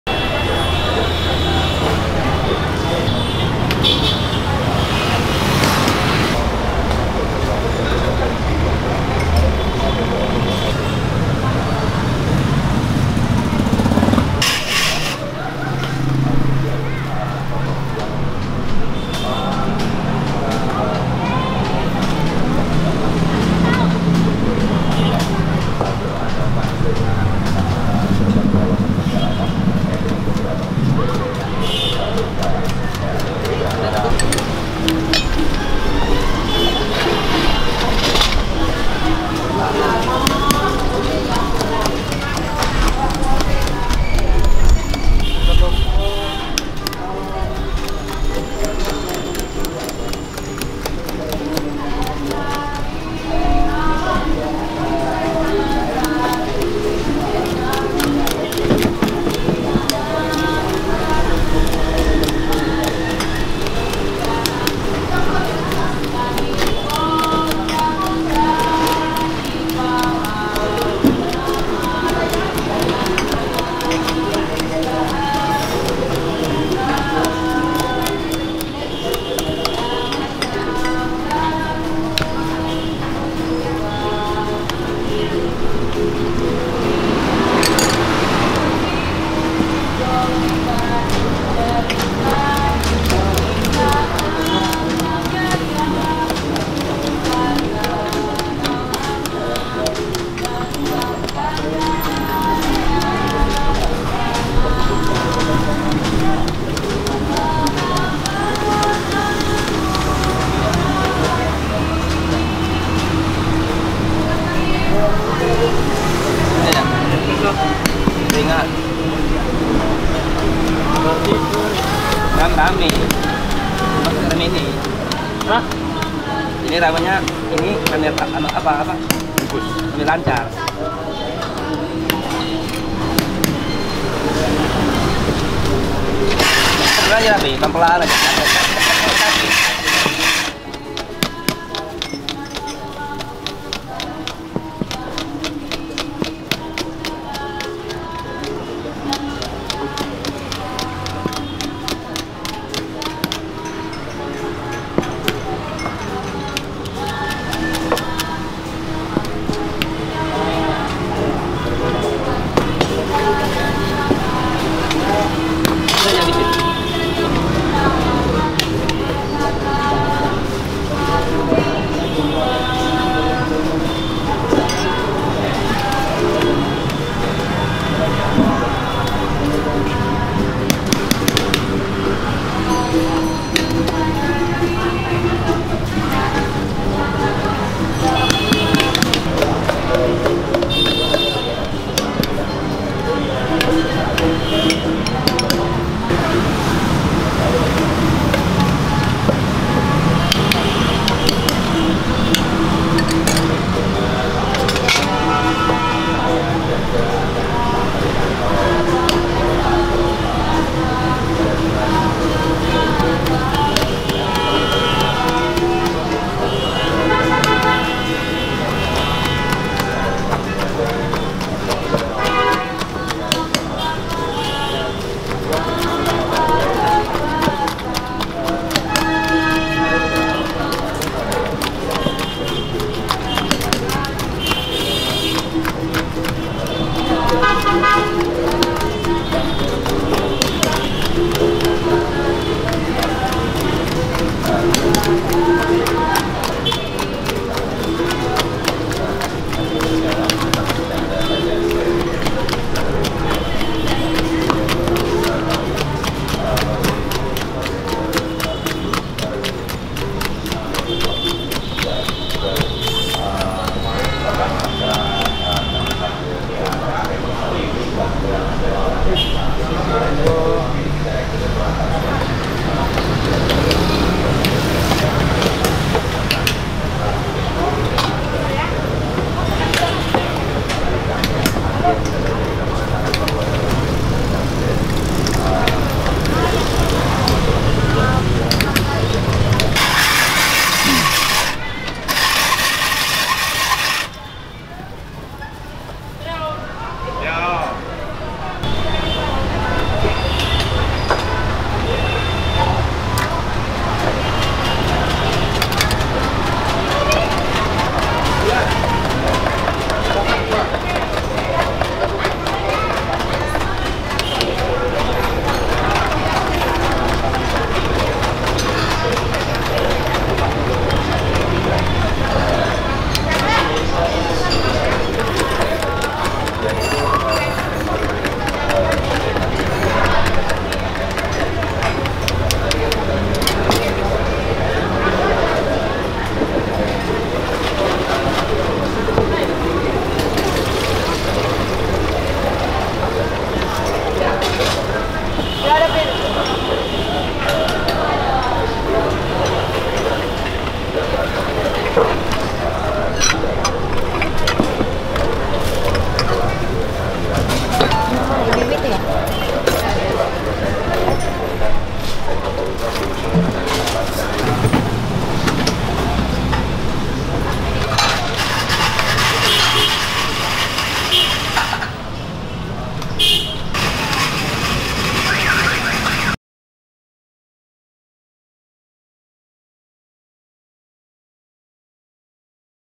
Avocado Shake Without Blender | Pokat Kocok Simpang Glugur | Indonesian Street Food
Avocado Shake / Alpukat Pokat Kocok Medan Simpang Glugur Buk Iyah / Mashed Avocado Smoothie / Medan Street Food / Indonesian Street Food